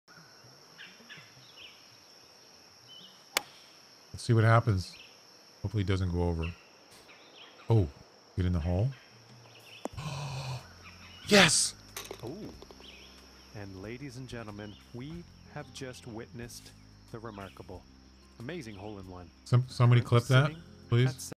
Ace on #8 Hazeltine in Tournament
This was on Hole #8, Round 3 - #1 Stroke Index hole